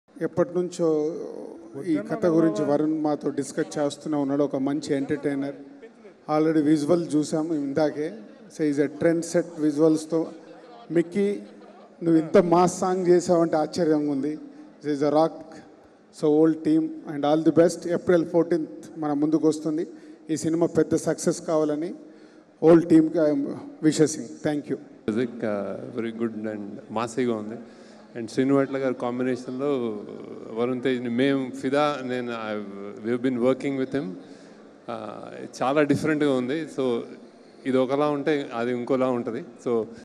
Dil Raju Speech At Mister Movie Pre-Release Function || Varun Tej, Lavanya Tripathi, Habah Patel
Watch Dil Raju Speech At Mister Movie Pre-Release Function.